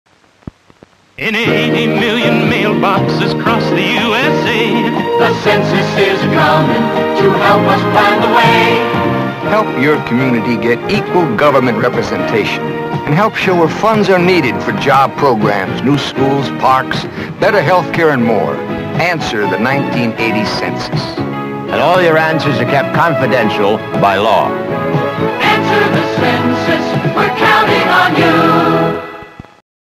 1980 Census Public Service Announcement - with Kirk Douglas
1980 Census Public Service Announcement - with Kirk Douglas - from a 16mm print